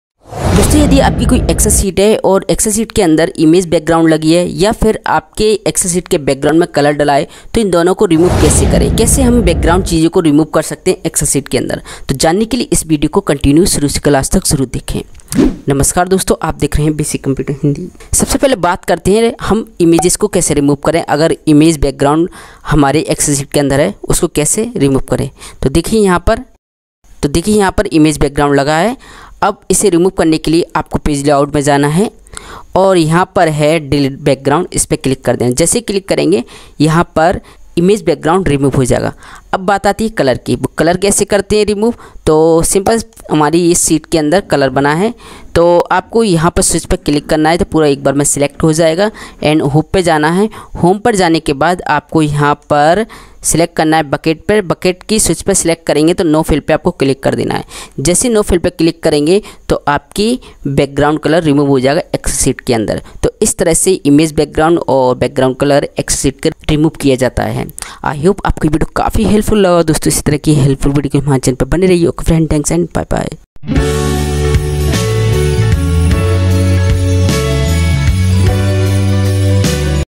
Remove Background Color & Image In Excel | Delete Background Color & Image In Excel
Namaskar dosto es video me humne bataya hai excel me background color & Background Image Remove kaise kare kya hai tarika excel ke andar background remove ya delete karne ka tou janne ke liye es video ko shuru se lekar last tak dekhe.